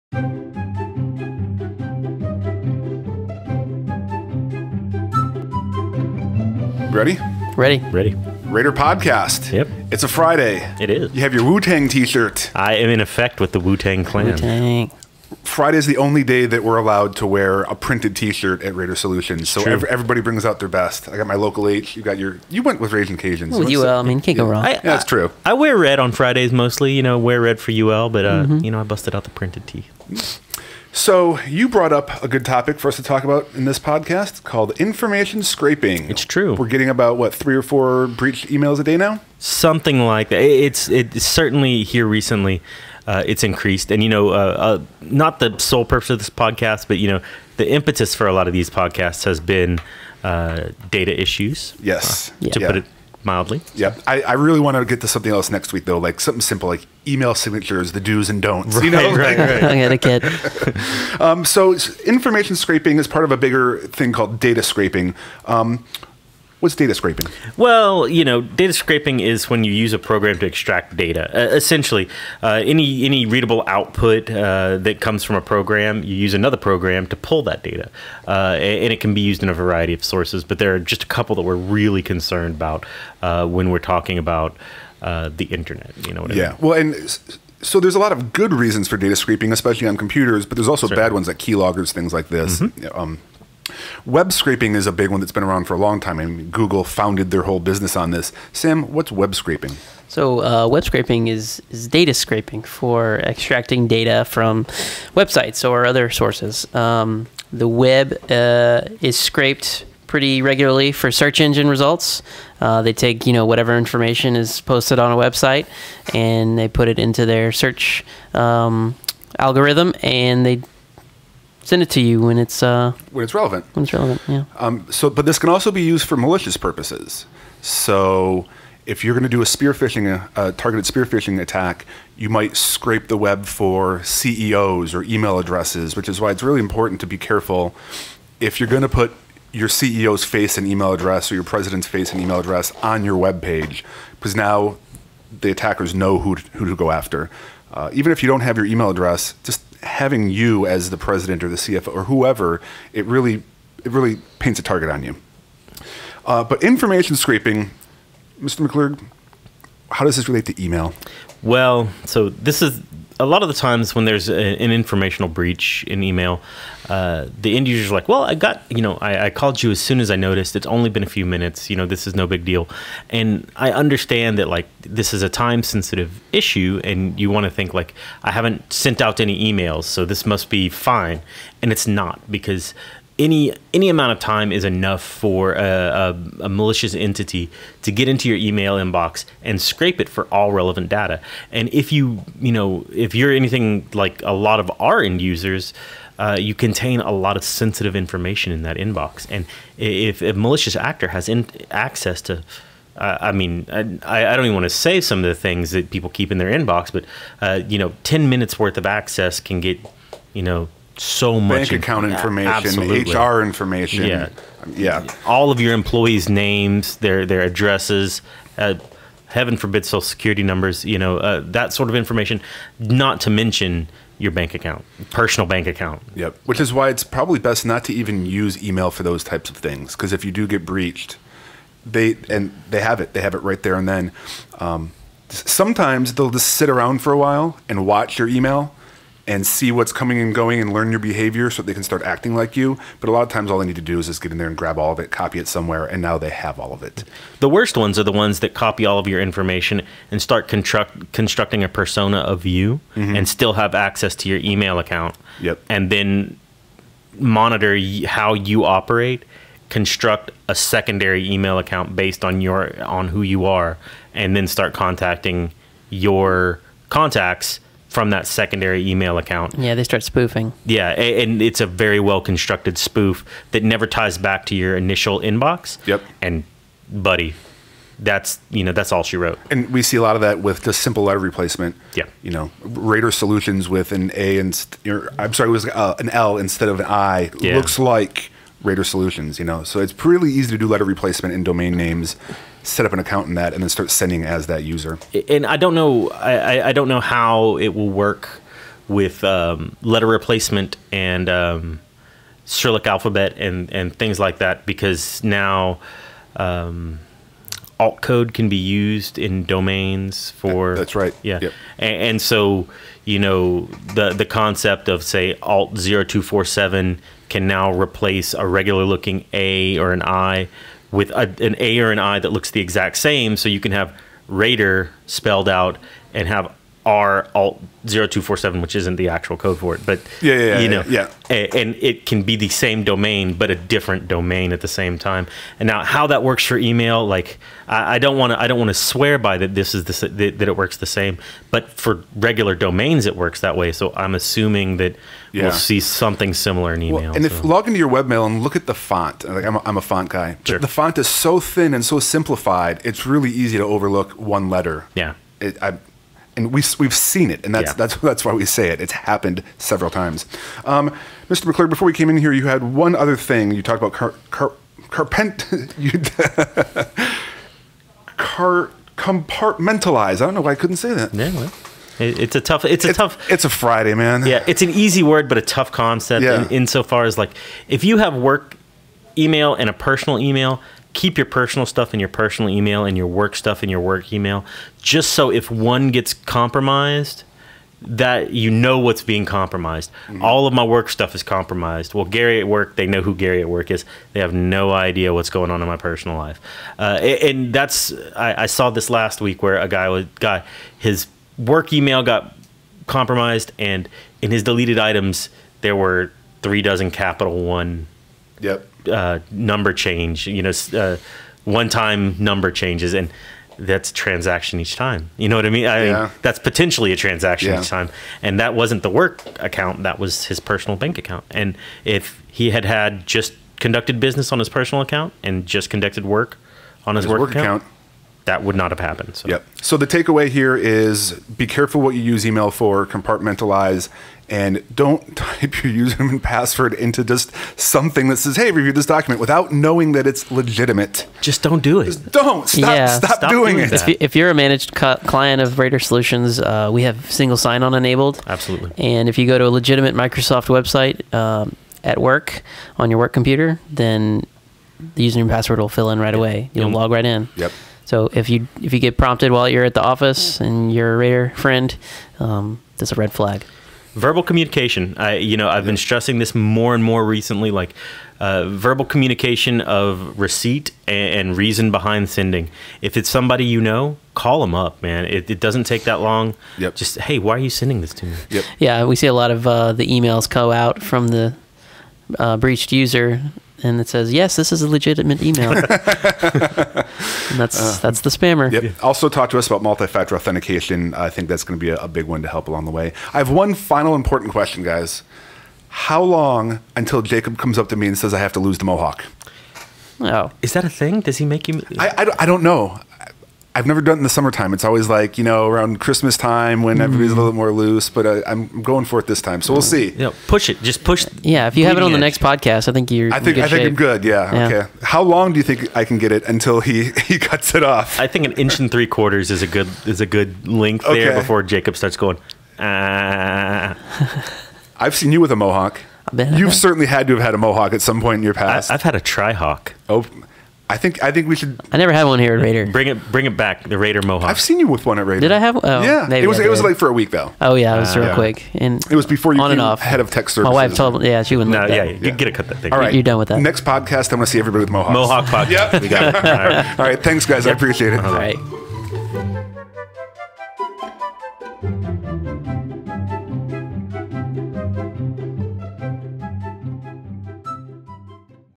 Rader Podcast - Data Scraping
In this video, we discuss data scraping and email account breaches.